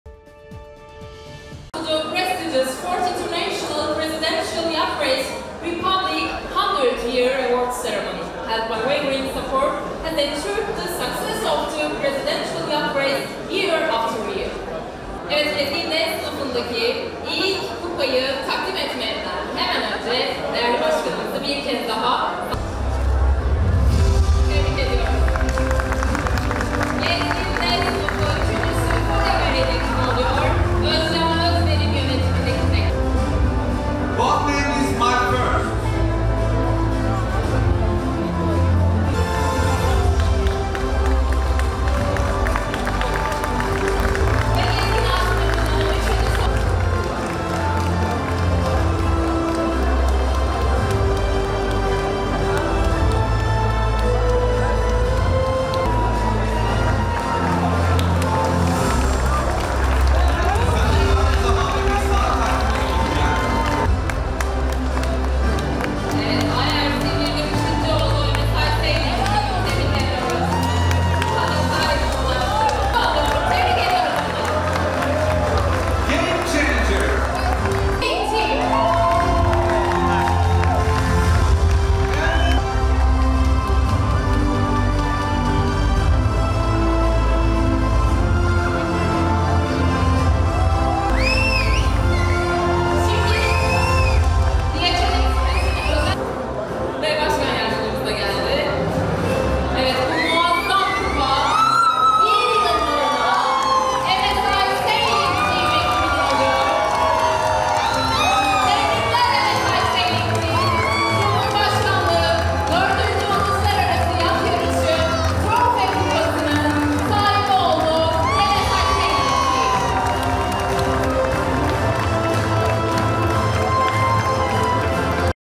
Cumhurbaşkanlığı 4 Uluslararası Yat Yarışları'nın ödül töreni düzenlendi
Cumhurbaşkanlığı 4. Uluslararası Yat Yarışları kapsamında düzenlenen Cumhuriyet 100. Yıl Kupası'nın ödül töreni gerçekleştirildi.
Cumhurbaşkanlığı himayesinde, Kültür ve Turizm Bakanlığı ile Gençlik ve Spor Bakanlığının katkılarıyla, İstanbul Valiliği iş birliğinde, Anadolu Ajansının  global iletişim ortağı olduğu, İstanbul Açıkdeniz Yat Yarışı Kulübünce organize edilen, DHL Express ana sponsorluğunda düzenlenen organizasyonun ödülleri Fişekhane'de yapılan törenle yarışmacılara takdim edildi.
MSI Sailing Team, Cumhuriyet 100. Yıl Kupası'nın sahibi oldu. Ahmet Köroğlu'nun kaptanlığını yaptığı MSI Sailing Team, DHL Özel Ödülü'nü de almaya hak kazandı.